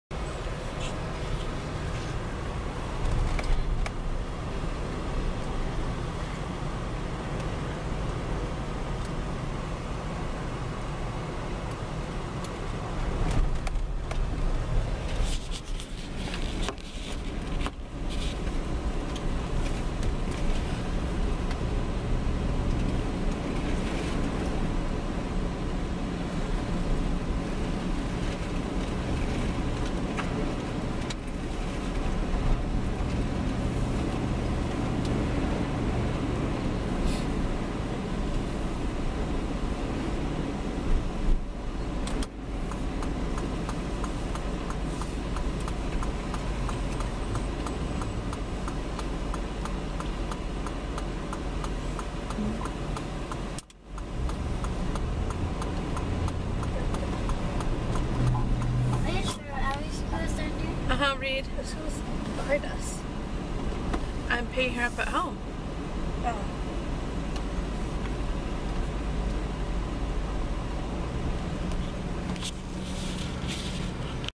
Minot, ND blizzard
Feb. 18, 2011  Just some snow for my NW friends who want some.  Be thankful you don't see this everyday.  LOL